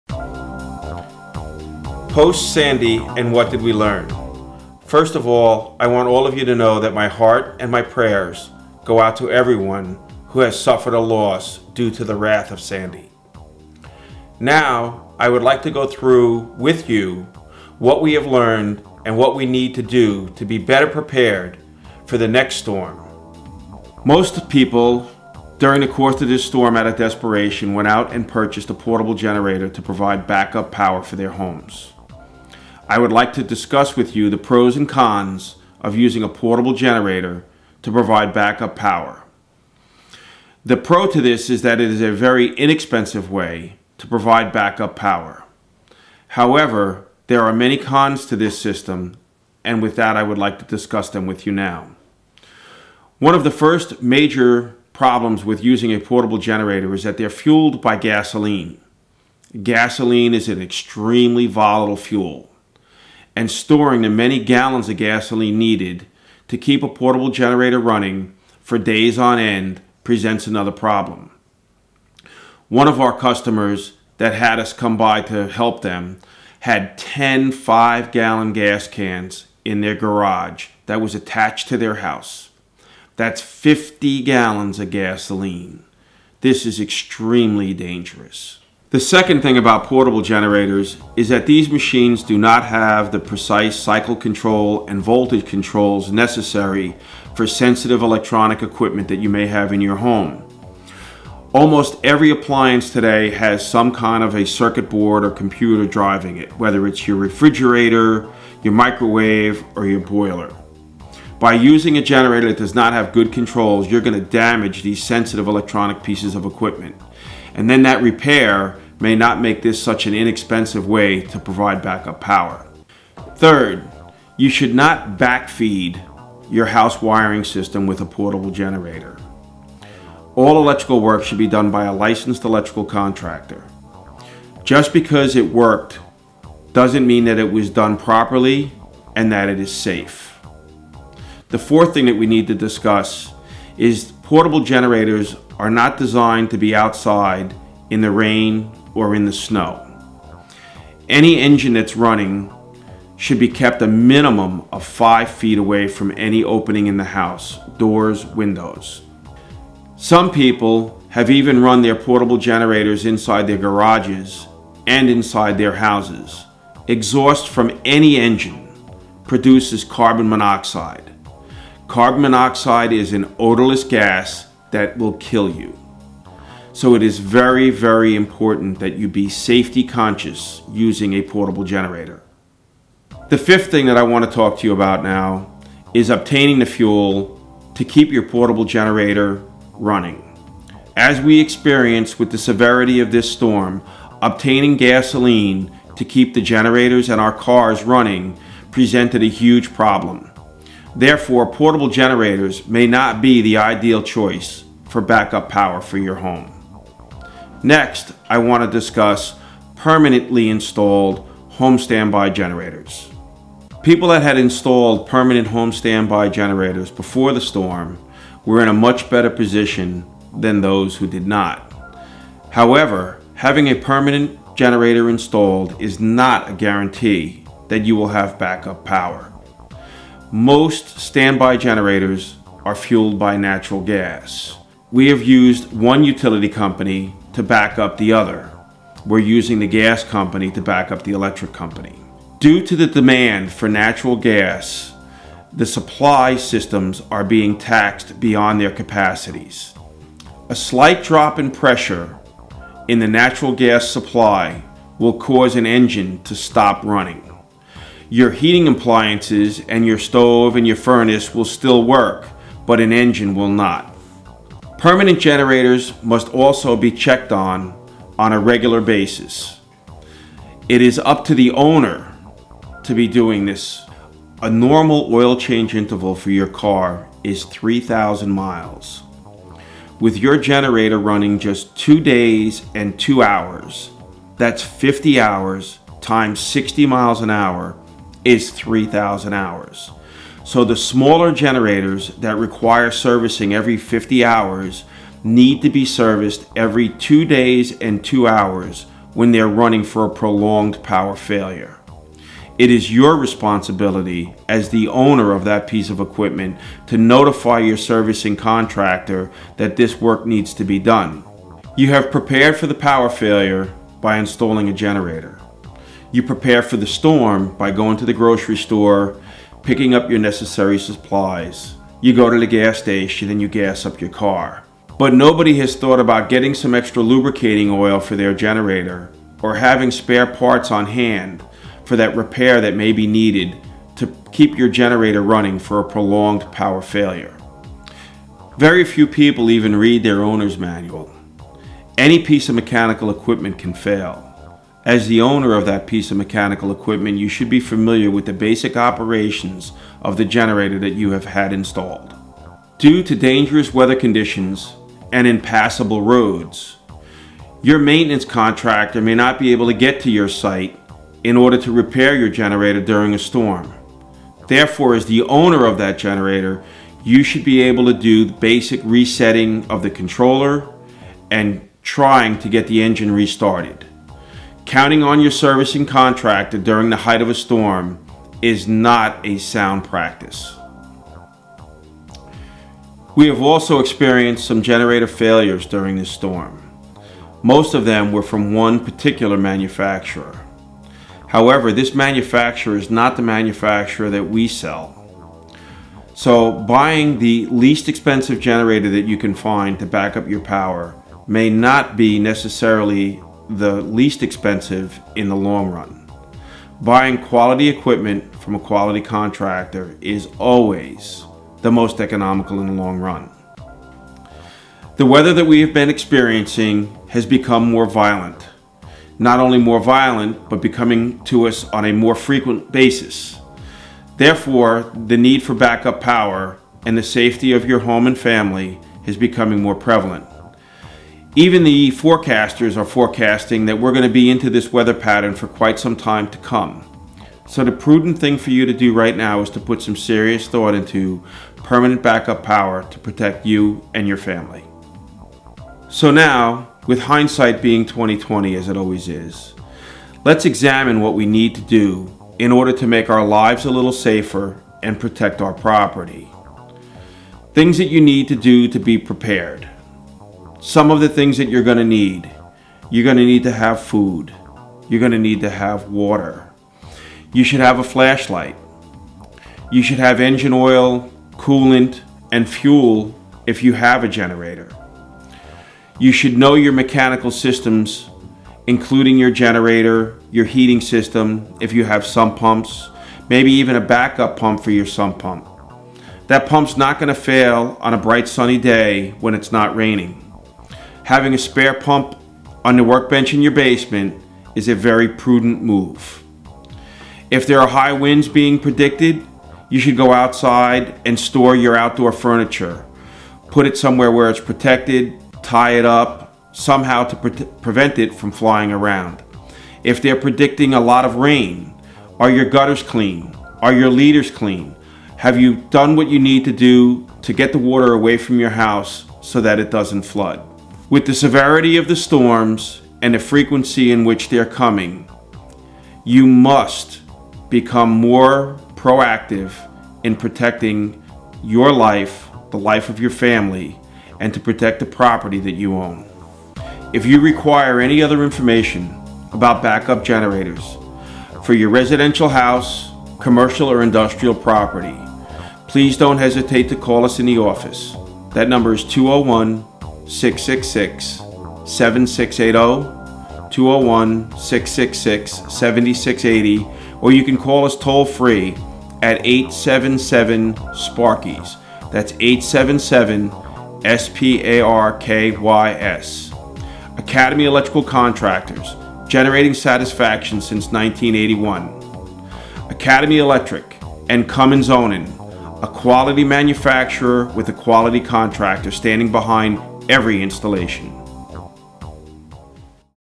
POST HURRICANE SANDY WHAT DID WE LEARN ABOUT GENERATORS?
Generator Dan talks about: POST HURRICANE SANDY
WHAT DID WE LEARN about Generators?
FIRST I WANT TO SAY THAT MY HEART AND PRAYERS GO OUT TO EVERYONE THAT HAS EXPERIENCED ANY LOSS DUE TO THE STORM.
Many people purchased portable generators for emergency power use in their homes.
Lets examine the pros and cons of portable generators Pro: it is a very inexpensive way to provide back-up power.CONS: There are many problems doing this. Some are down rite dangerous.